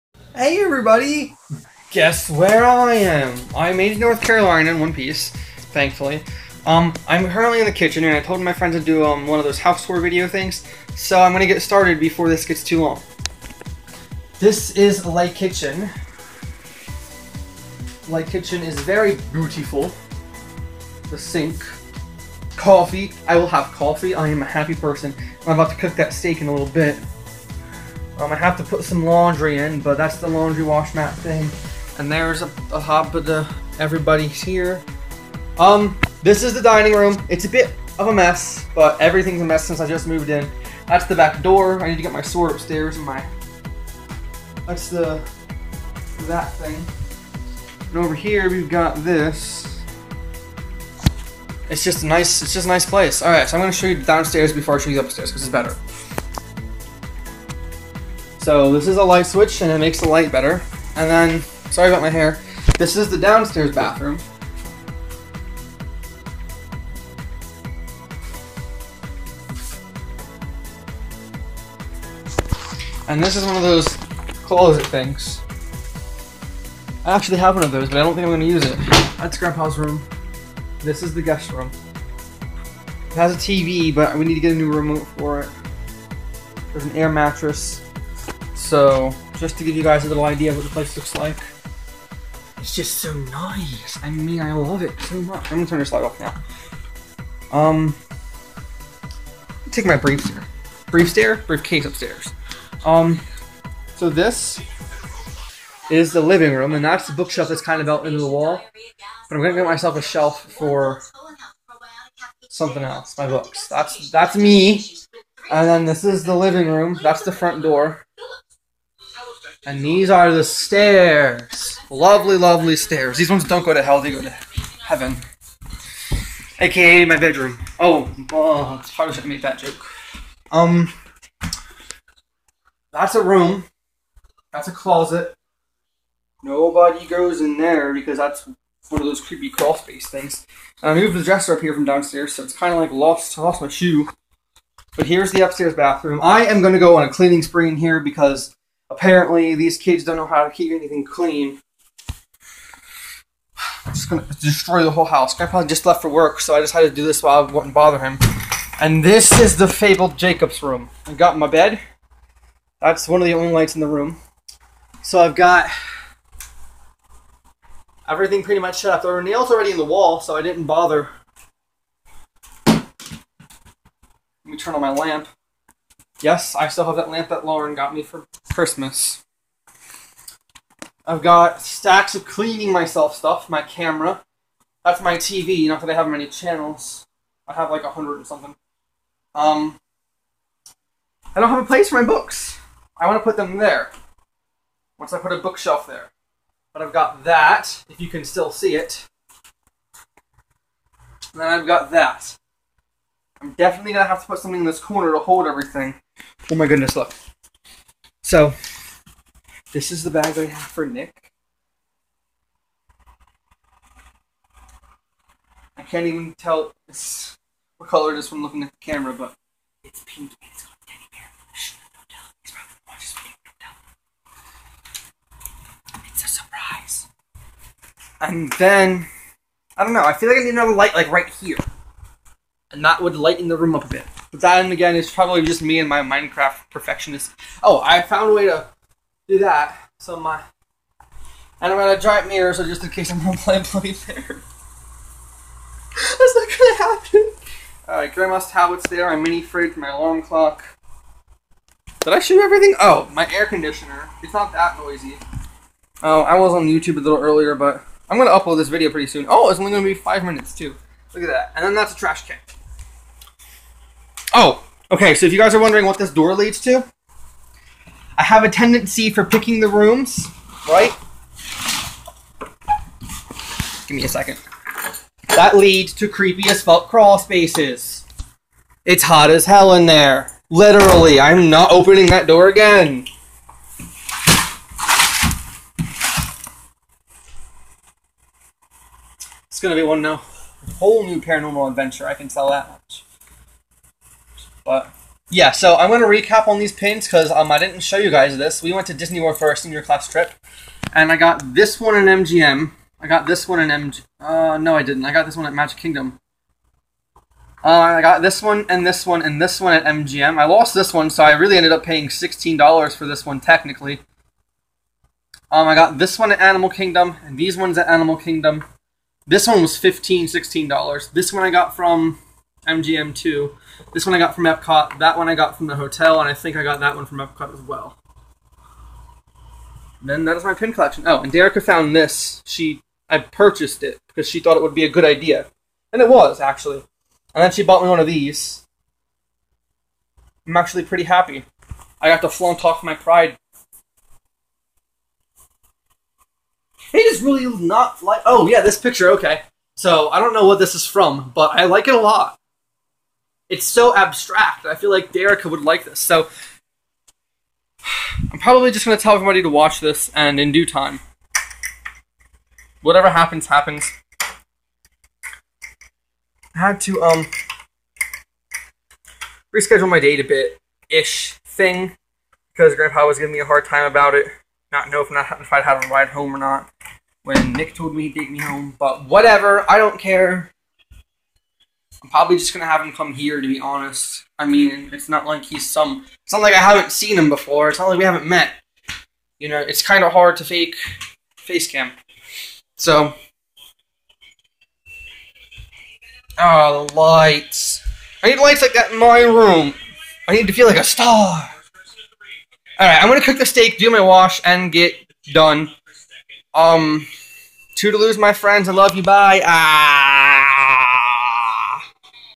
House Tour!!!
Recently moved and this is what I did to show you my new home. Love you guys so much. Thanks for all of the support.